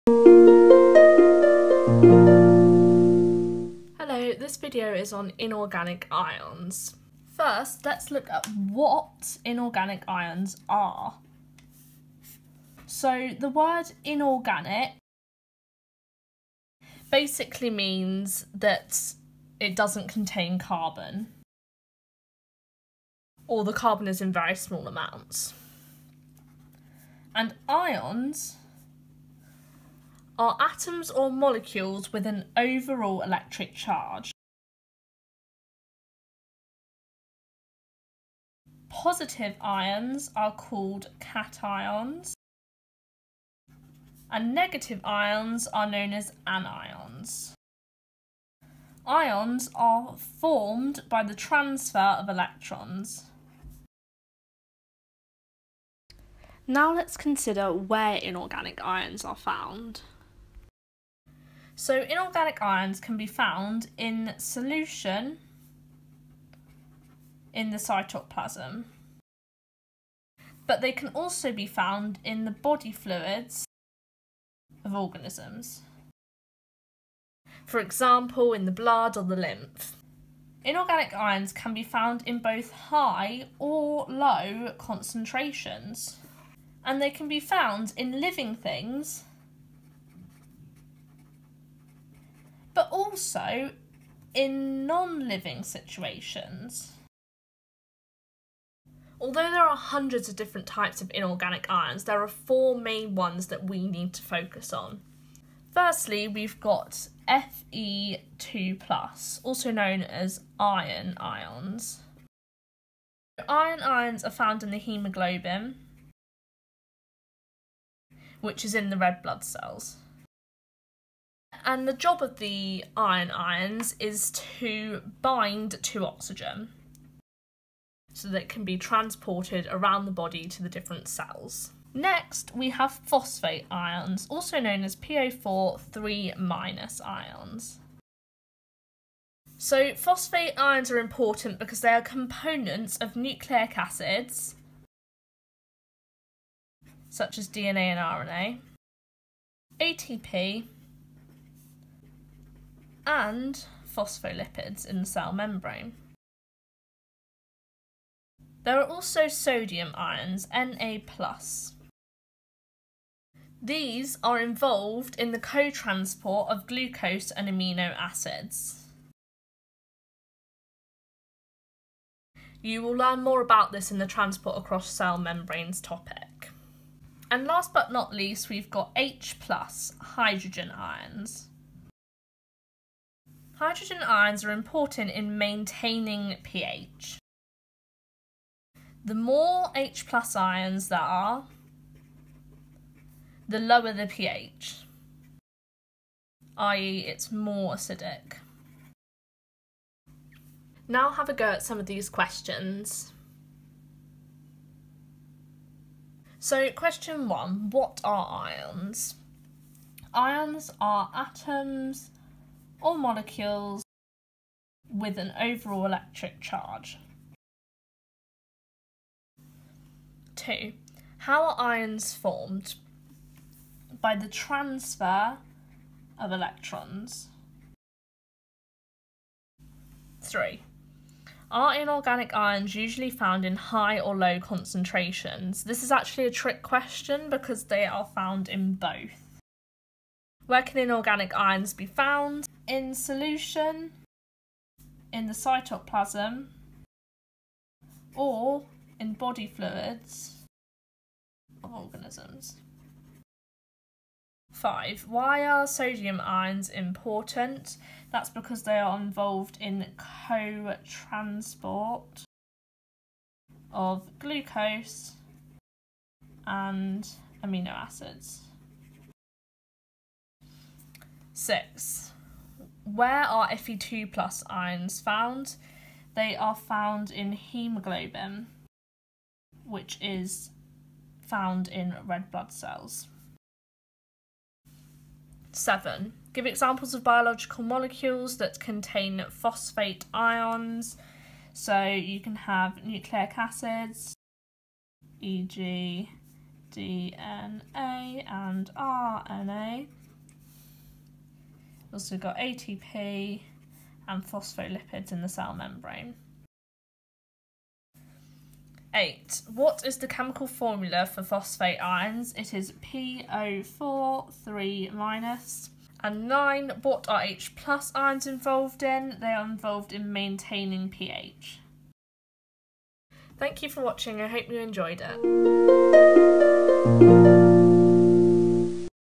3.1.8 Inorganic Ions Revision | AQA A-Level Biology
This short revision video tells you what inorganic ions are and where they can be found. Hydrogen ions, iron ions, sodium ions and phosphate ions are looked at in more depth.